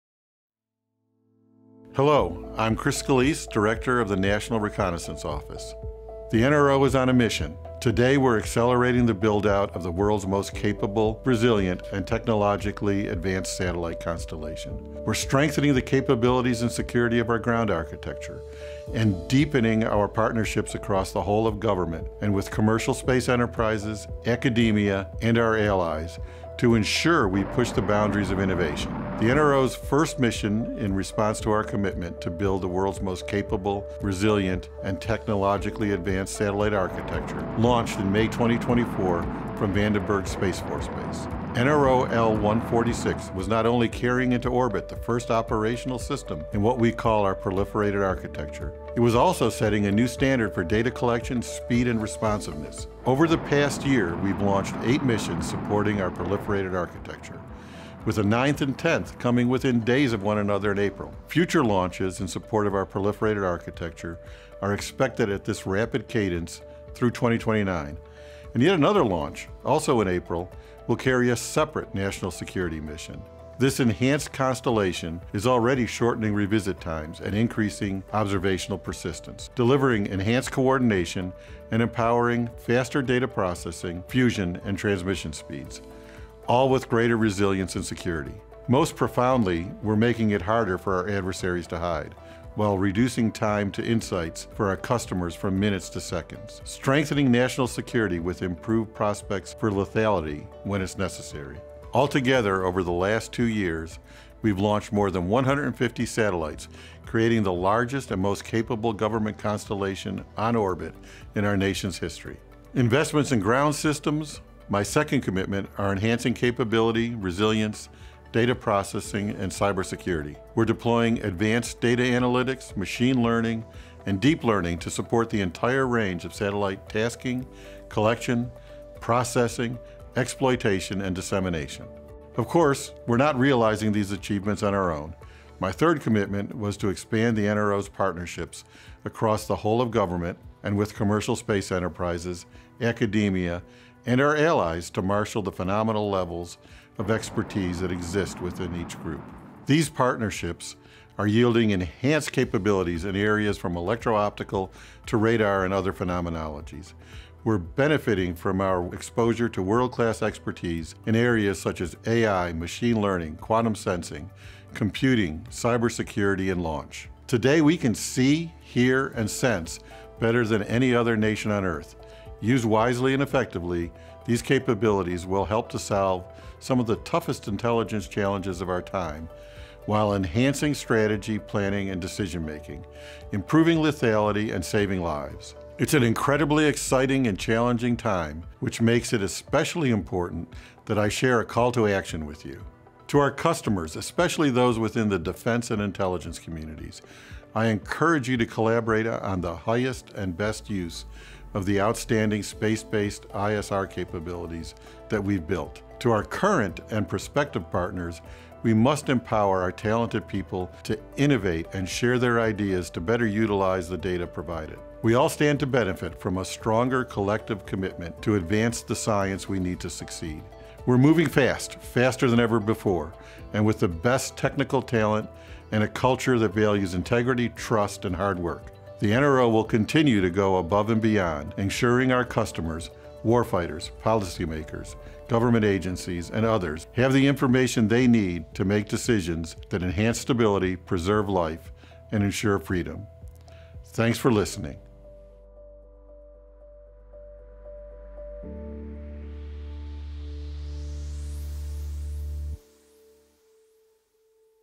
NRO director calls for leveraging full potential of cutting edge space capabilities
One year ago, the NRO made several public commitments to ensure that our nation remains ahead of the national security challenges that we face.

Listen to Director Scolese’s update to find out how the NRO has met – and in many cases, exceeded – those objectives, and what Scolese is calling on the community to do next.